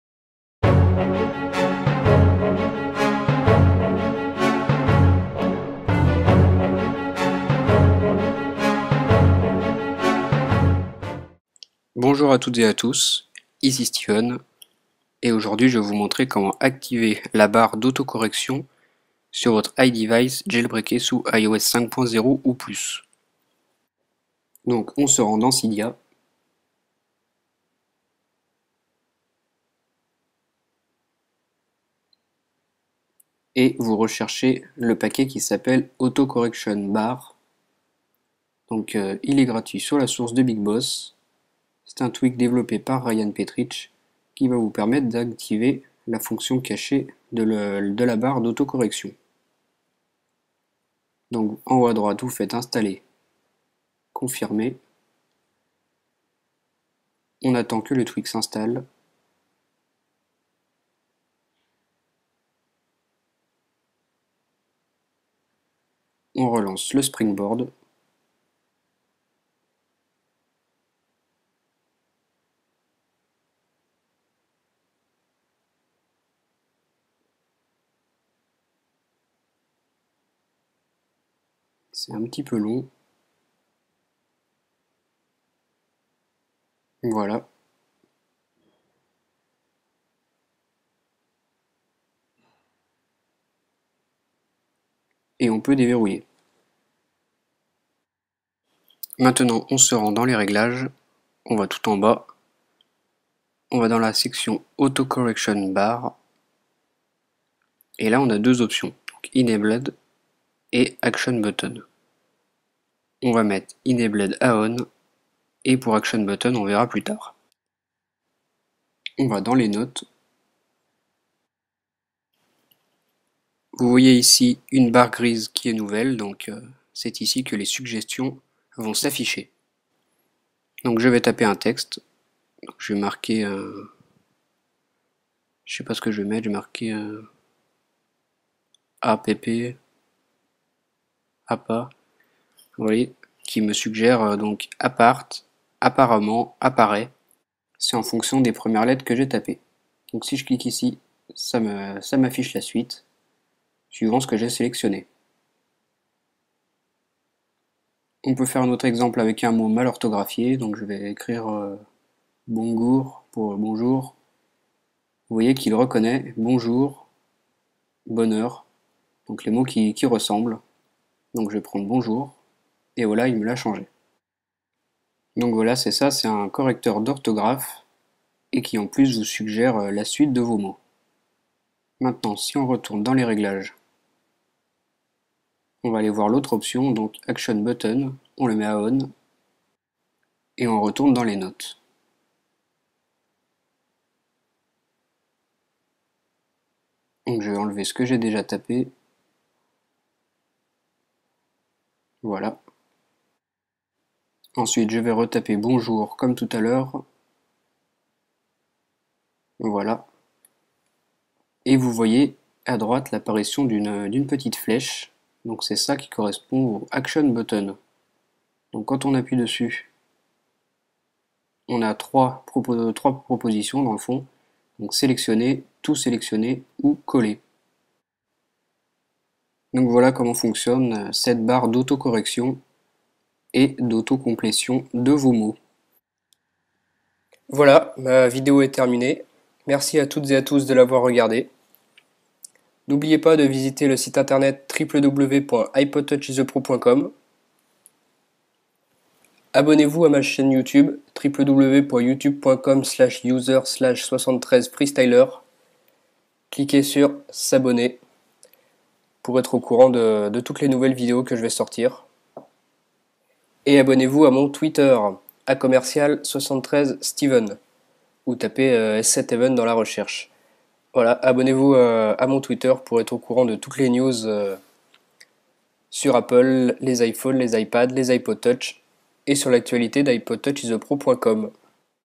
AutocorrectionBar - Activer le mode de correction sur iOS 5 (iPhone / iPod Touch / iPad)
Voici AutocorrectionBar, un tweak Cydia qui vous permet d'activer la fonction de correction d'orthographe non activé par Apple mais présente dans le code d'iOS 5